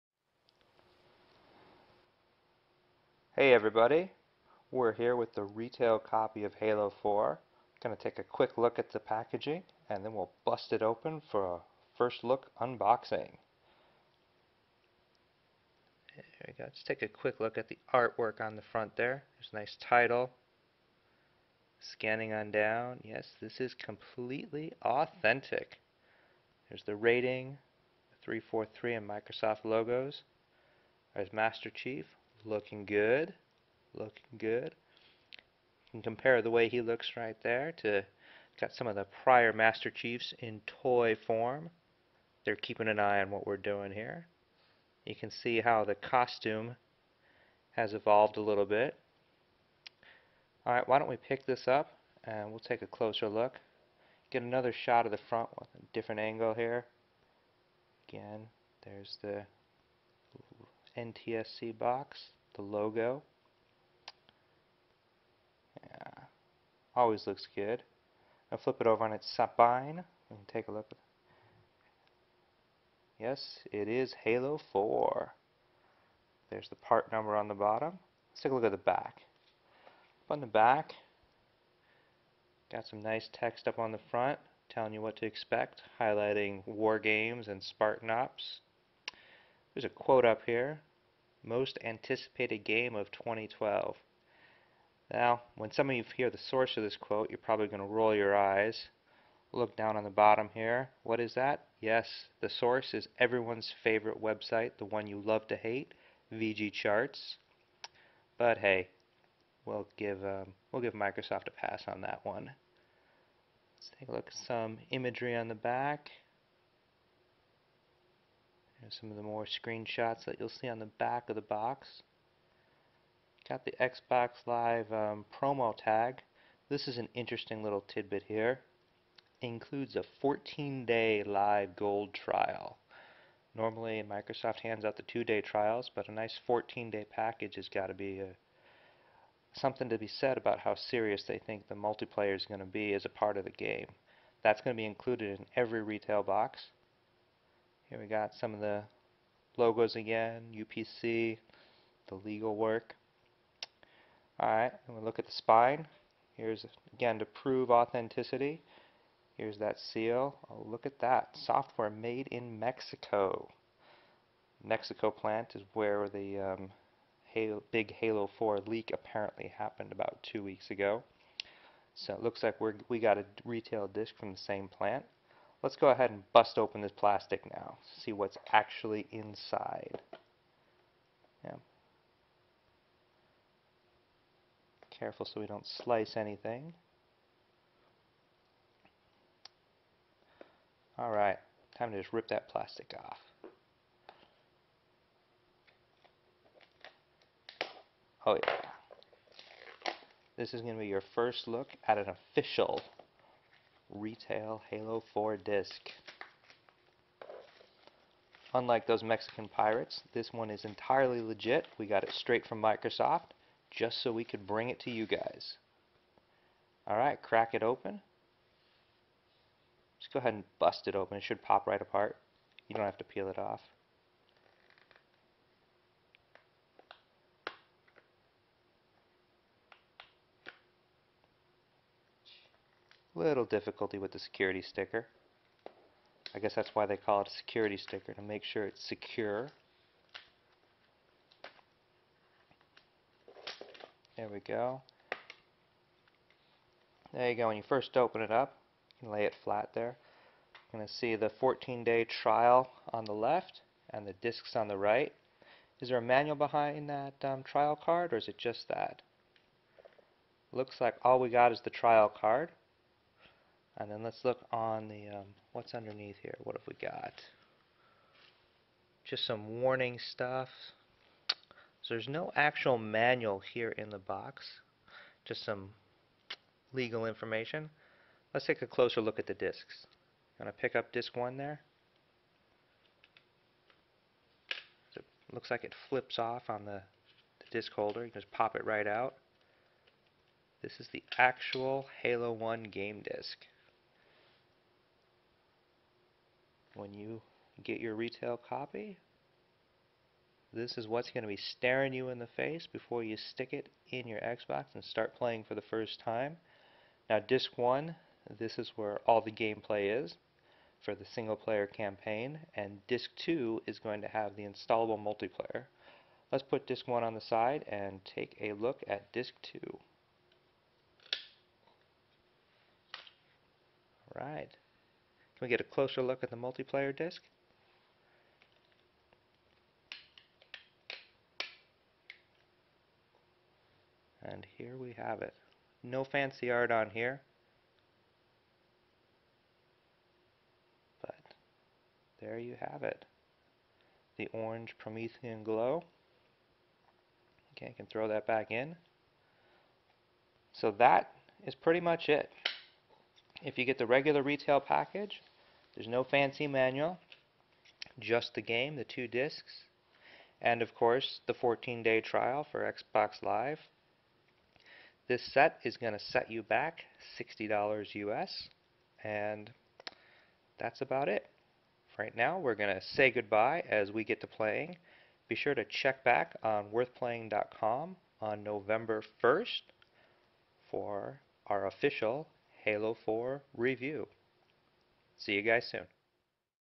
Halo 4 Standard Edition Xbox 360 Unboxing
Get a sneak peek at the retail edition of Halo 4. We take a quick look at what's inside the box so you know what to expect when you get your copy.

The standard edition of the game comes with two game discs and a 14 day Xbox Live Gold trial subscription code.

Halo 4 is scheduled for release on November 6, 2012.

on November 1, 2012 for a full review of the game.